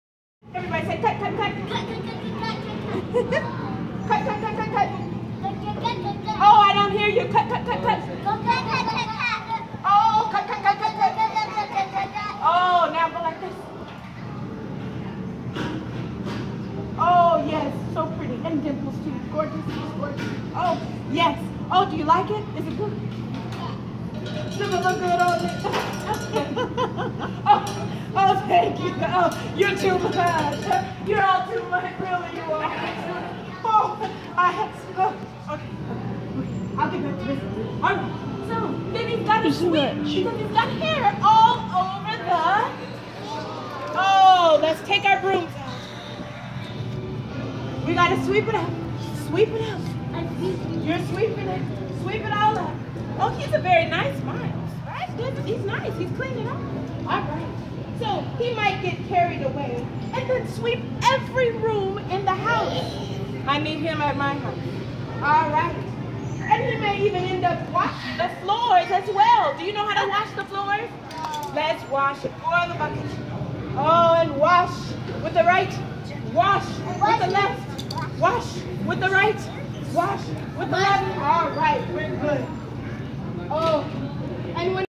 "If You Give a Mouse a Cookie" clip 2 by LACA Bridges
Storytelling clip taken at Children's Museum of Ft Worth (Ft. Worth Museum of Science and History), Storyland exhibit